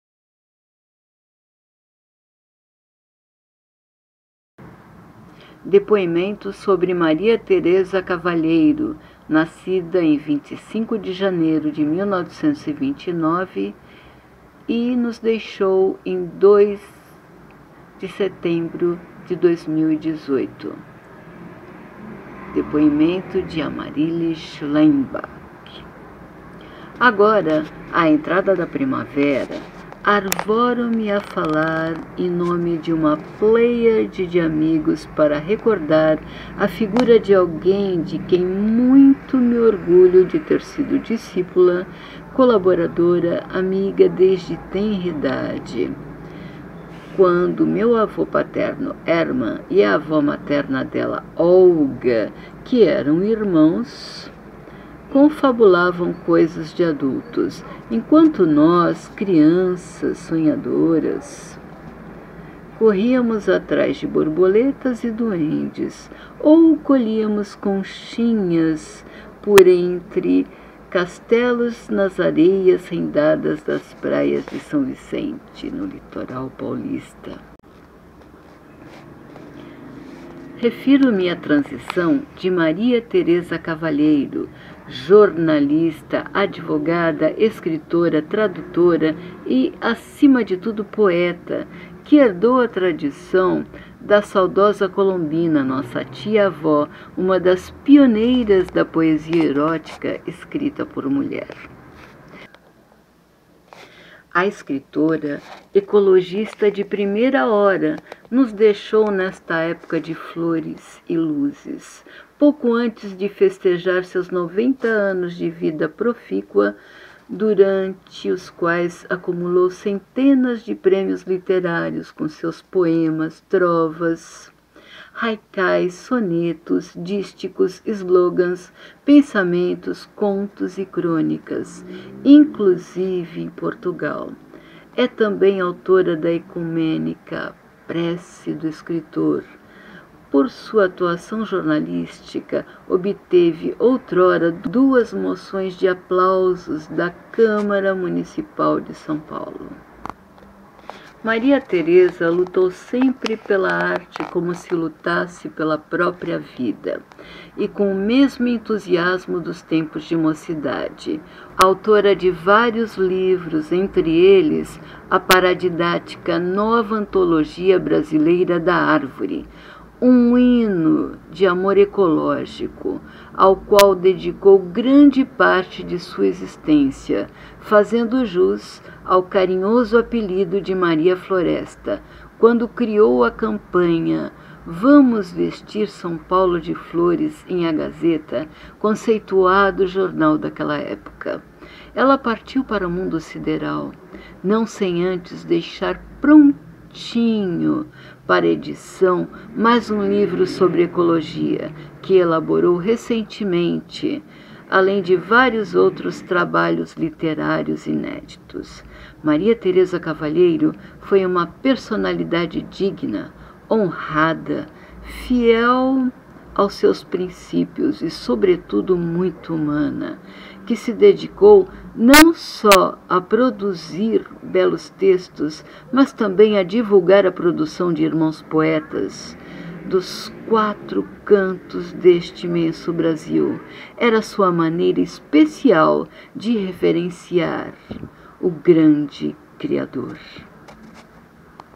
Depoimento sobre Maria Thereza Cavalheiro de Amaryllis Schloenbach
Homenagem de sua prima e amiga.